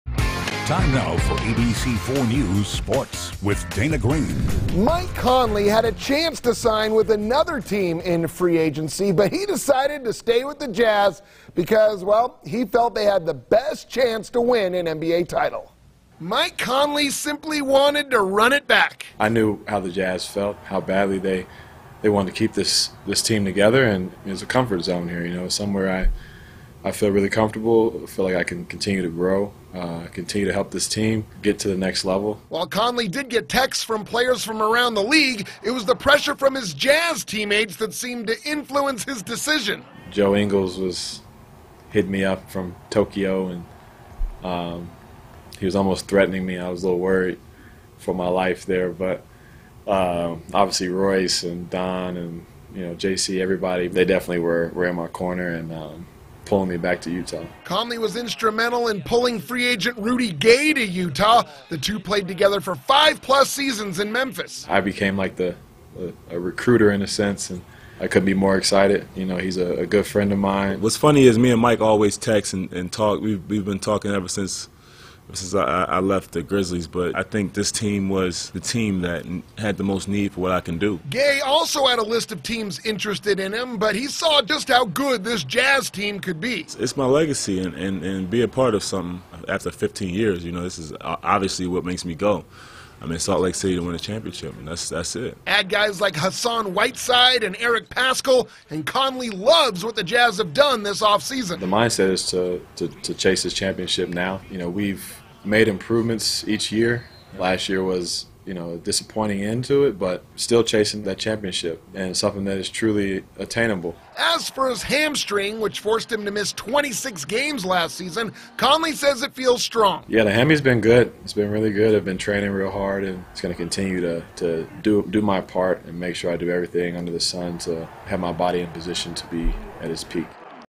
Mike Conley wants to win a title in Utah
Conley re-signs with Jazz